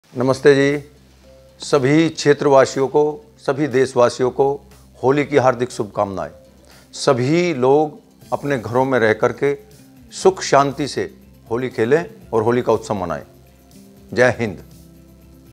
शामली सदर विधायक प्रसन्न चौधरी ने दी होली की बधाई | AB STAR NEWS
About ABStarNews